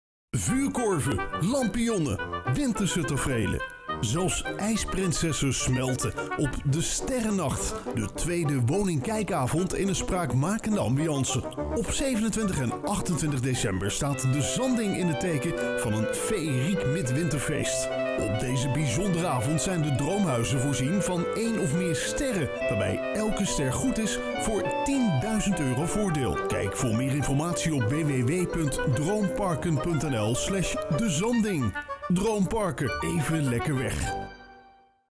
Sterrennachten op DroomPark De Zanding
Op 27 en 28 december a.s. staat DroomPark De Zanding volledig in het teken van de Sterrennachten.